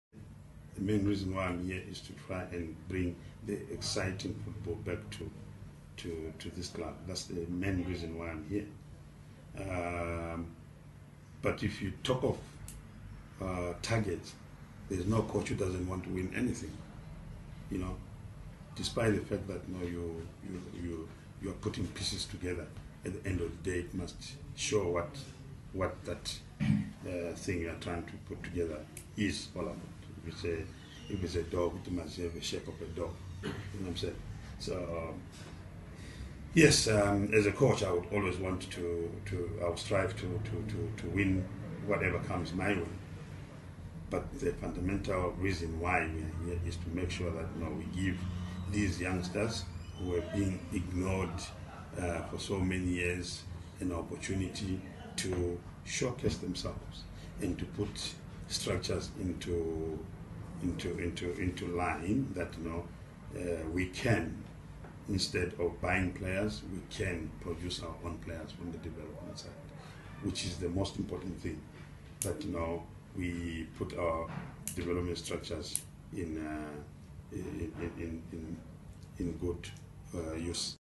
Madinda Ndlovu: I Want to Succeed Like Any Coach
Highlanders head coach Madinda Ndlovu says like any coach in the world, he wants to win games to ensure that Bosso succeeds as much as possible. (Video: Joseph Njanji)